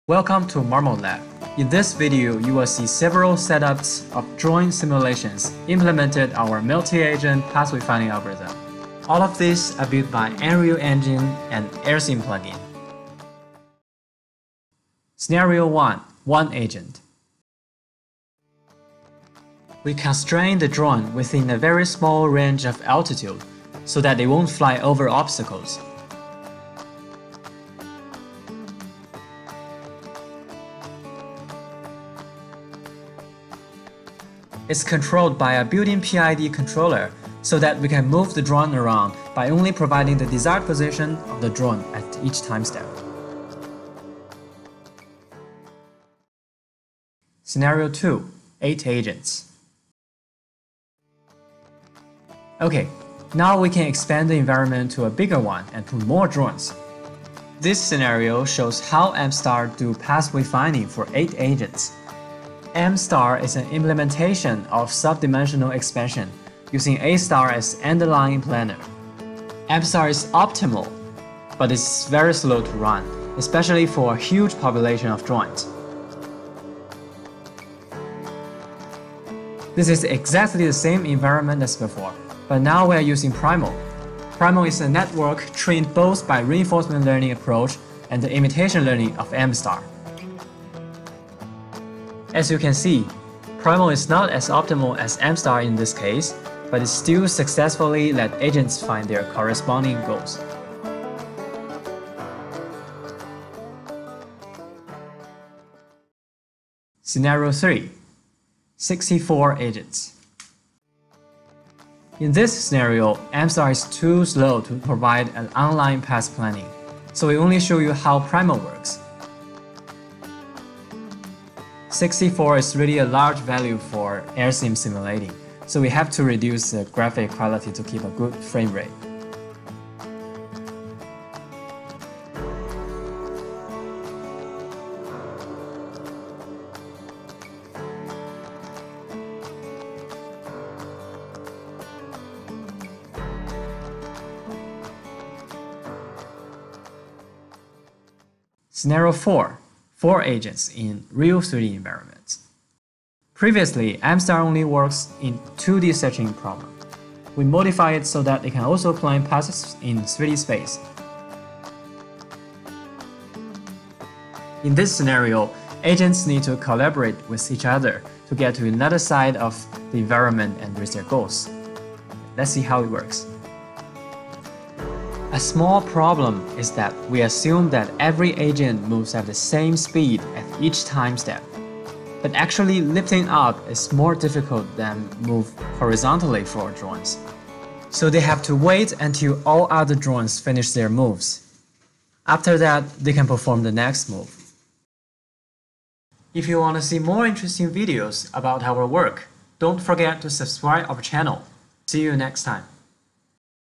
Deep reinforcement learning based multi agent pathfinding
AirSim simulation results from the MAPF controllers developped in the ME5001 (master's) project "Deep reinforcement learning based multi-agent pathfinding" by Zhiyao Luo.

In this video, we setup various 2D and 3D scenarios to illustrate how PRIMALc and 3D-M* plan collision-free paths for multiple UAVs. These drones are controlled directly by position commands, and their altitude is considered constant in the 2D cases. Note how the drones can move smoothly along the discrete path point controlled by built-in controller without any collisions. 2D cases are solved with the AI-based, decentralized path planner PRIMALc, described in the associated report, while the 3D cases show results from a 3D extension of M* (since training of 3D-PRIMAL could not be completed due to time).